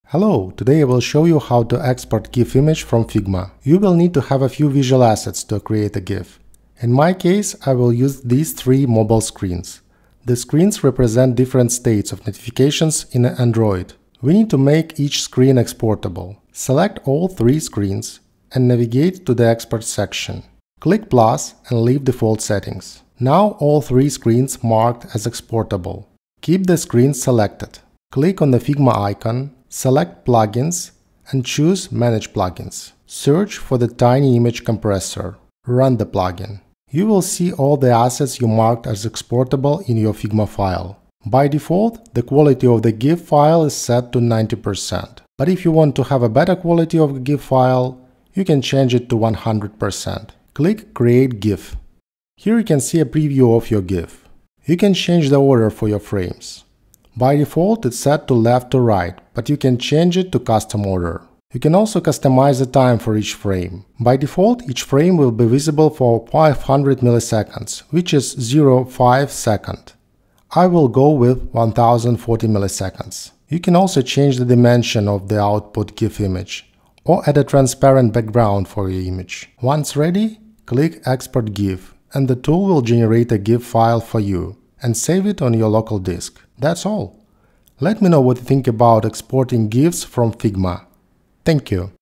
Export Figma Design To GIF
In this tutorial, I will show you how to export any visual design from Figma to a GIF file. This comes in handy when you need to create a demo for stakeholders or add a nice visual to your Dribbble or Behance portfolio.